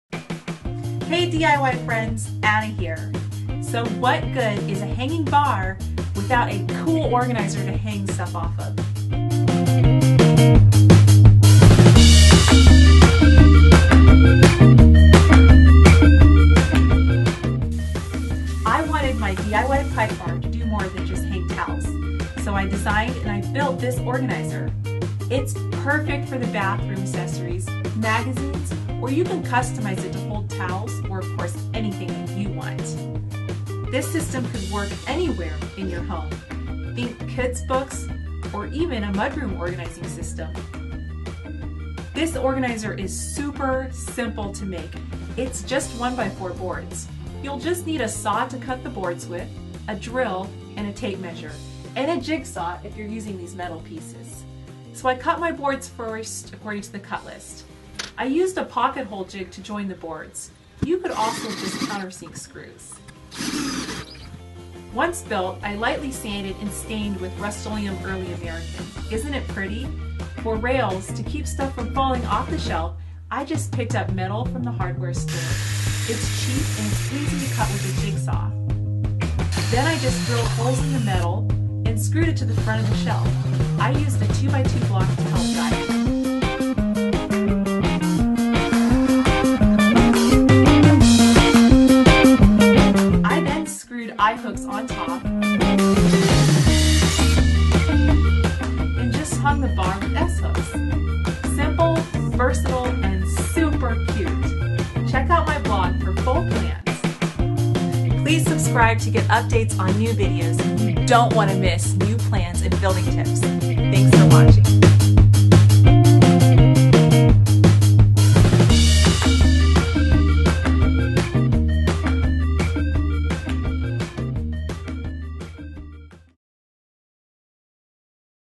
Industrial Hanging Organizer Tutorial
How to build a hanging organzier with metal rails. Easy DIY project you can make for under $20!